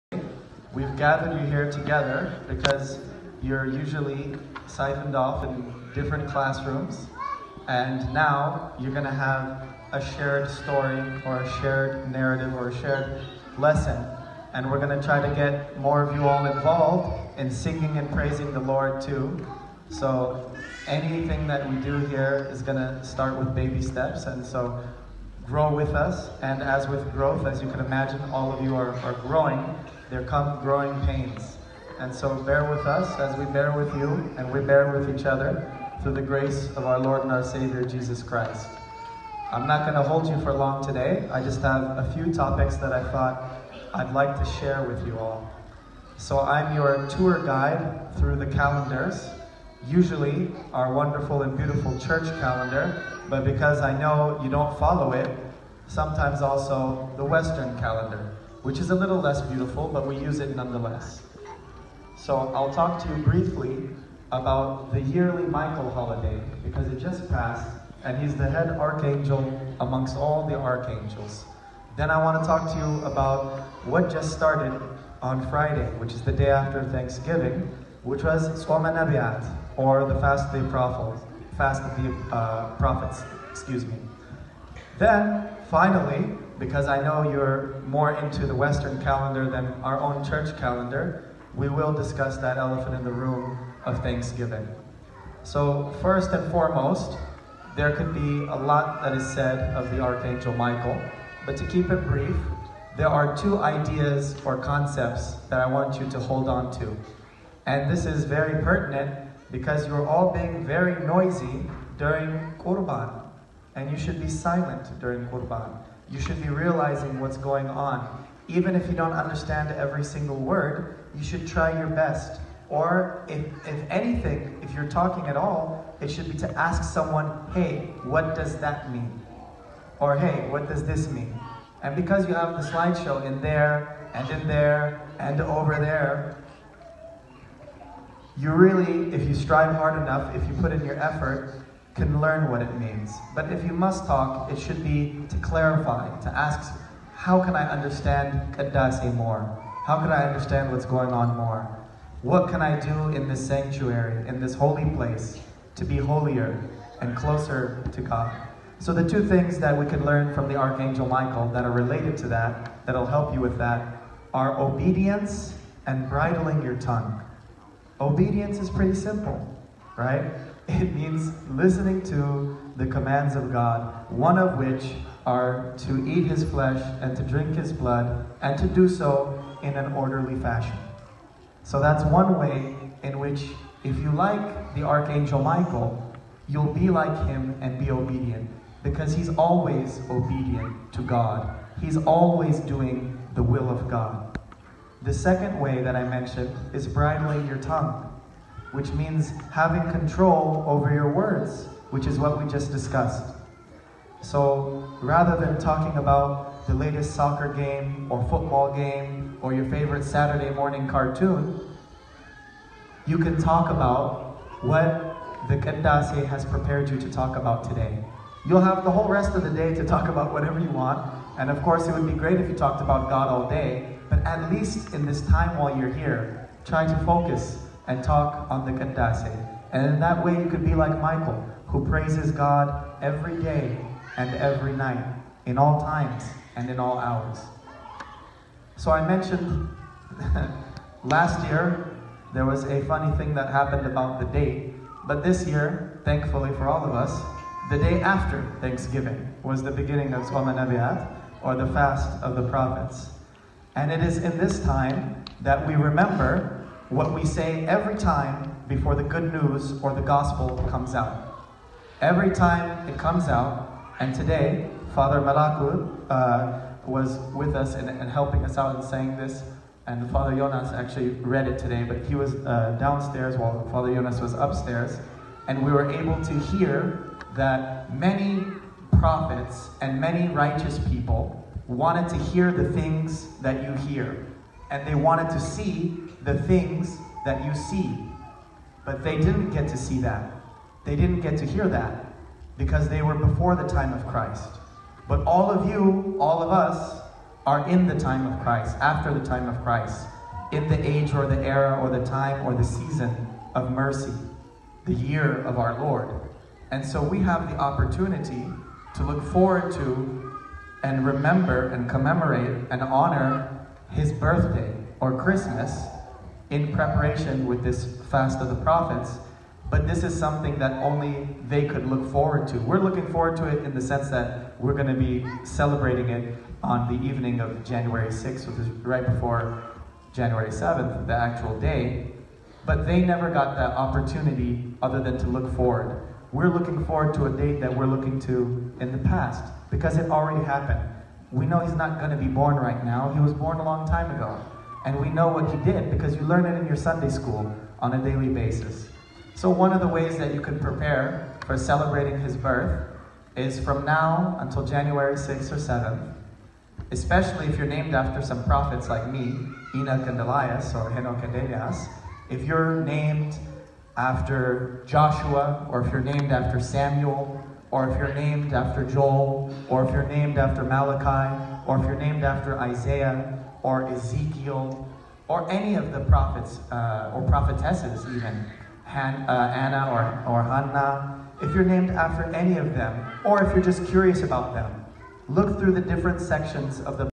Holy Michael & Prophet's Fast
Reflecting a new practice that began in ATL, under His Beatitude Bishop Yaiqob and His Beatitude Bishop Selama, Virgin Mary's EOTC (see of His Beatitude Bishop Bernabas) in South (Central) Los Angeles began implementing weekly homilies before the entire congregation in English around Dec. 2017. This is the first homily.